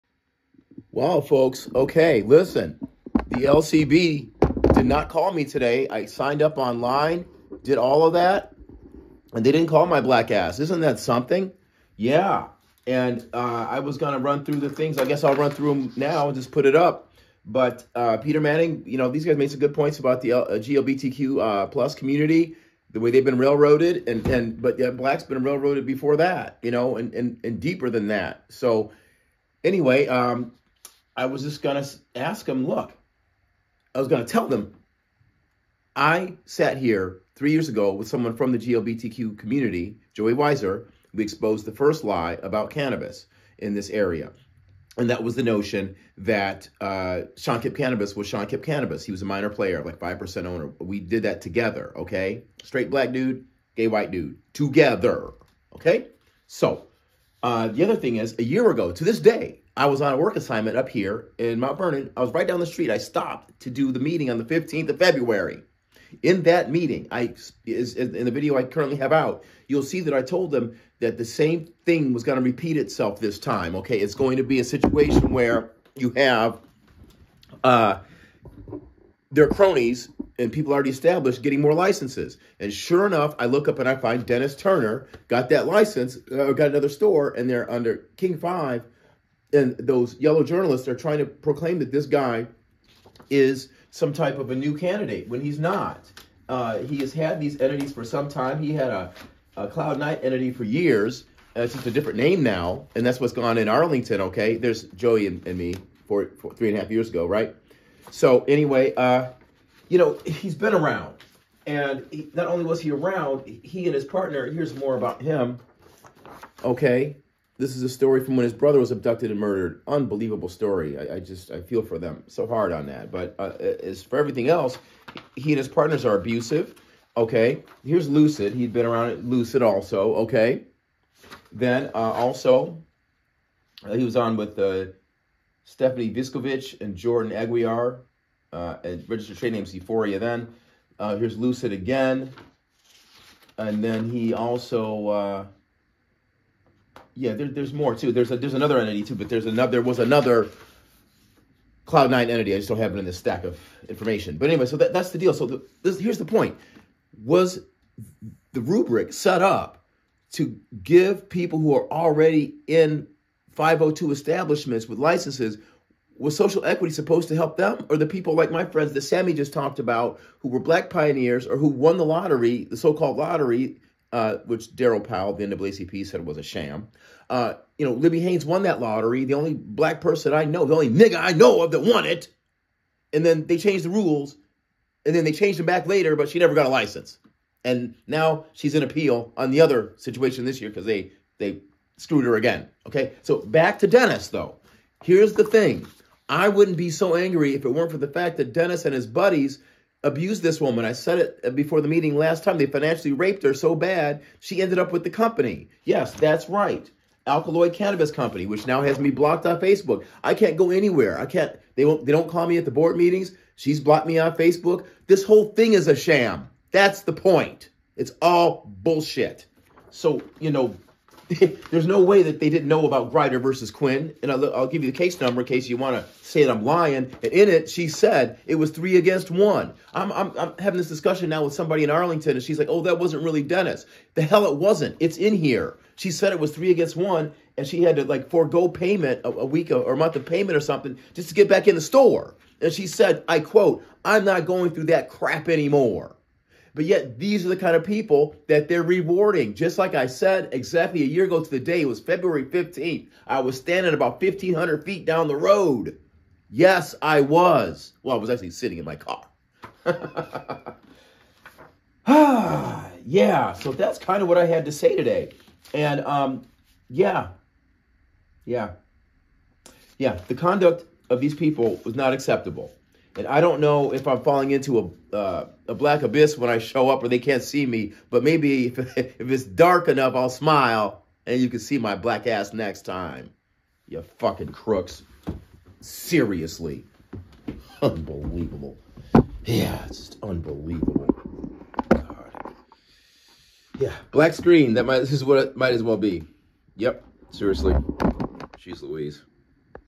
WSLCB Refused to Call me to Speak Because Cannabis Social Equity is a Joke
These people are so very evil that only an Atom Bomb will fix this. Here’s what I would have said, going to YouTube as well.
No way in the hell was the point of Social Equity supposed to be rewarding existing i502 stakeholders a chance to expand while others like my Friends Kevin Shelton and Libby Haines Marchel sit on the sidelines. And particularly ABUSIVE existing i502 stakeholders see generally Grider v. Quinn 37433-5-III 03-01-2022.

I had predicted this exactly one year ago today as noted in my last video when I stopped in Mt. Vernon in my car to catch the meeting.

And of course I have always worked with GLBTQ+ community as noted Joey Weiser - GWM - and I exposed the first lie about Black Cannabis in Seattle – “Shawn Kemp Cannabis” we exposed well before any major or other press did.
KingCast: Just the facts.